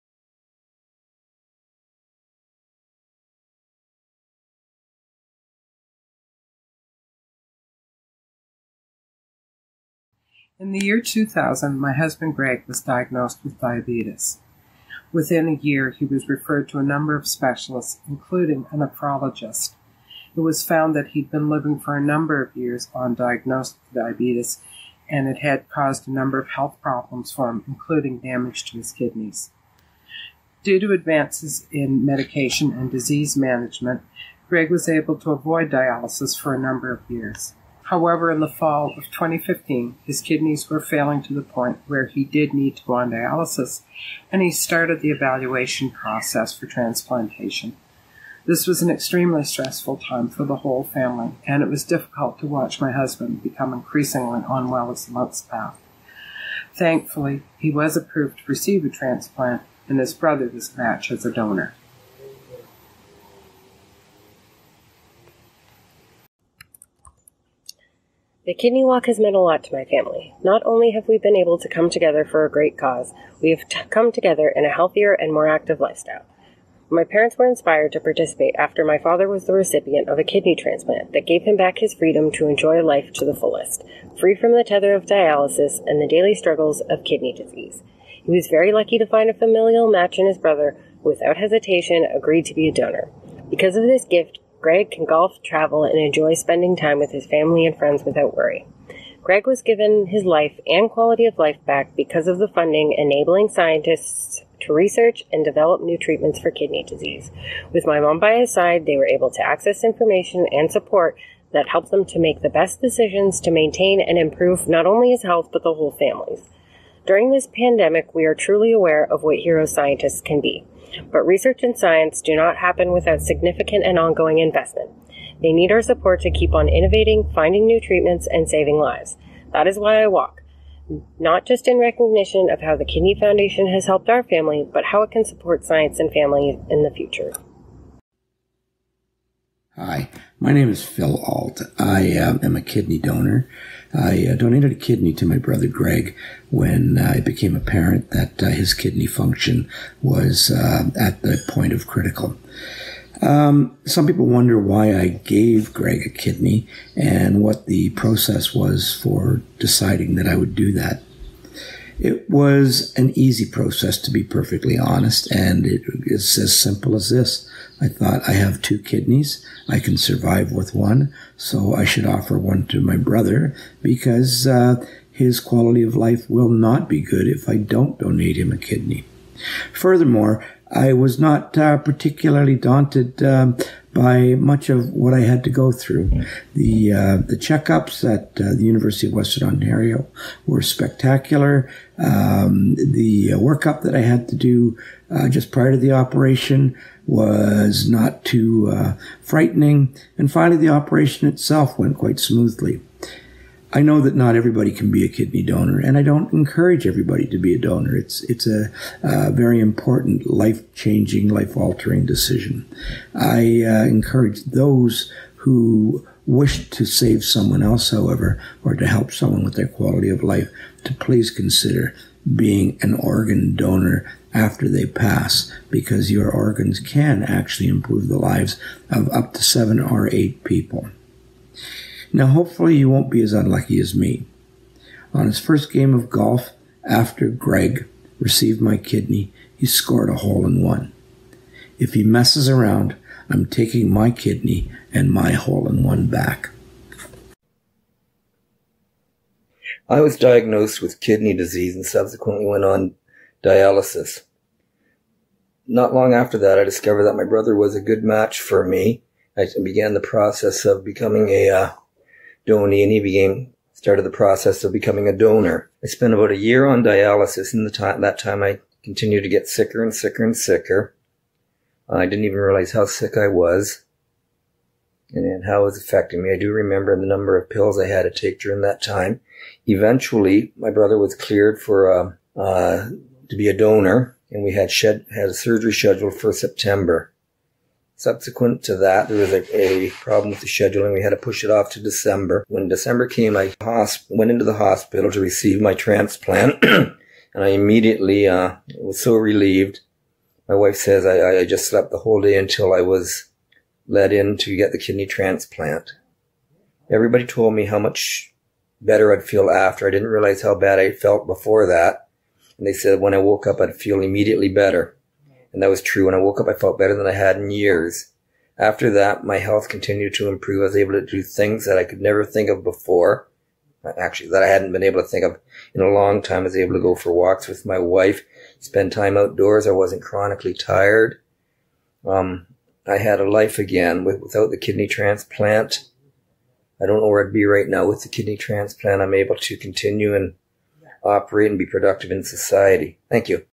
Team Allt's Kidney Walk message
Team Allt share their kidney message and the reason why they walk. Join them virtually this Sunday and show your loved ones you've got their back!